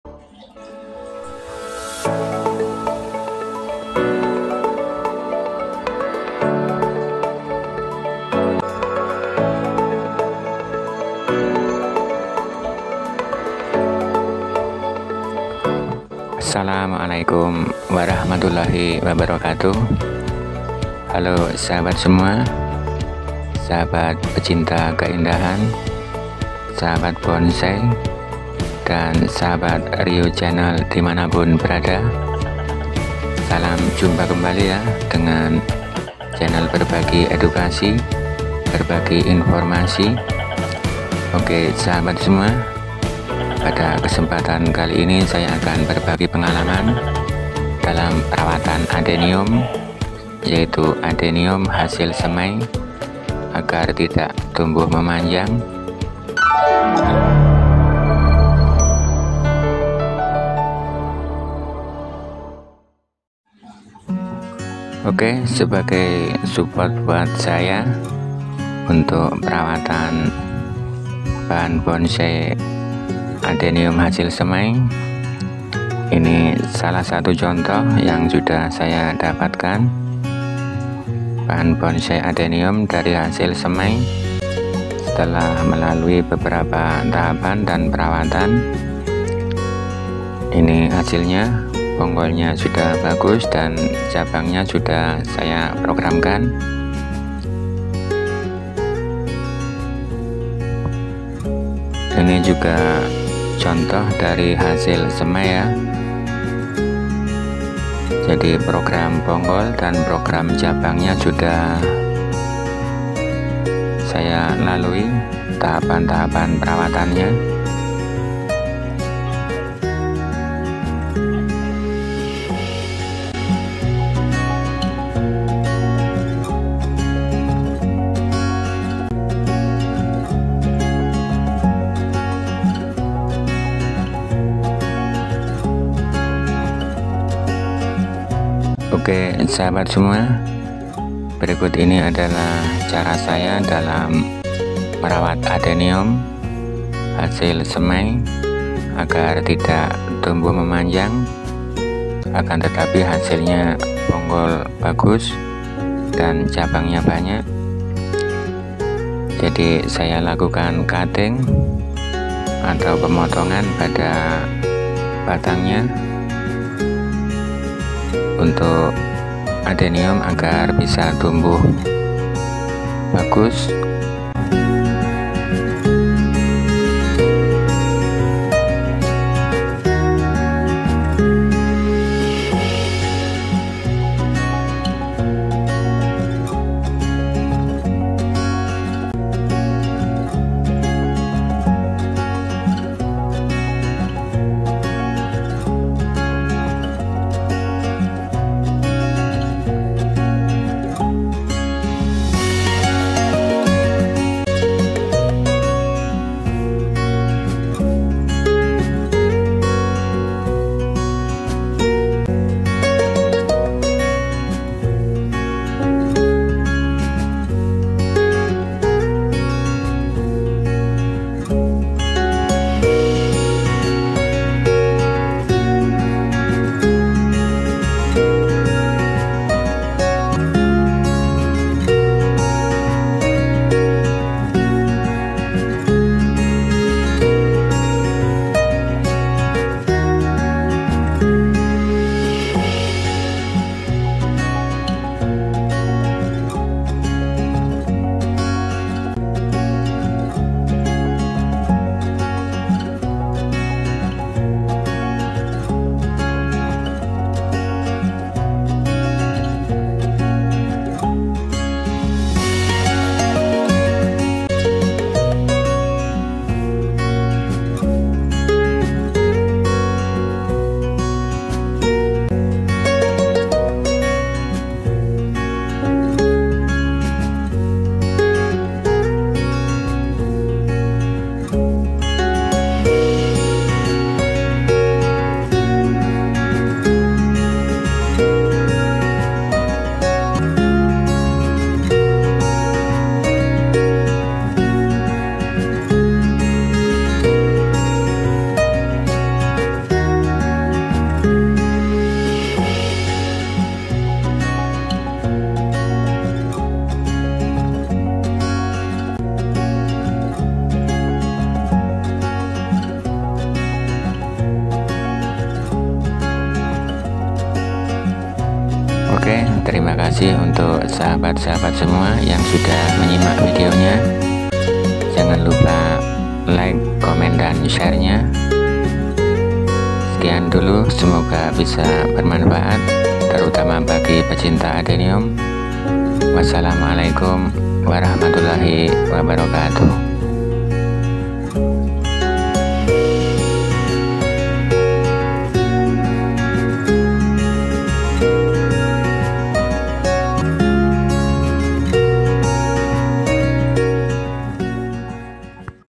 CARA MERAWAT ADENIUM SEMAI AGAR BONGGOL BESAR DAN TIDAK MEMANJANG
perawatan adenium semai agar bonggol dan cabang bagus adeniumbonsai @Riyo74-channel